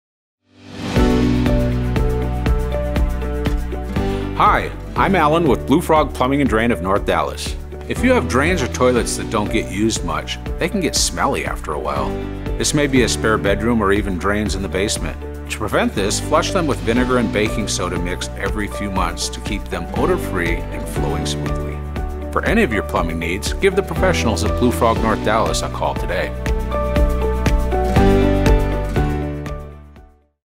Smelly Drains and Toilets | Plumbing Service North Dallas
Here are some tips for preventing your drains and toilets from getting smelly, especially in an area they aren’t used often.

For any of your plumbing and drain needs give us a call, we’re happy to help!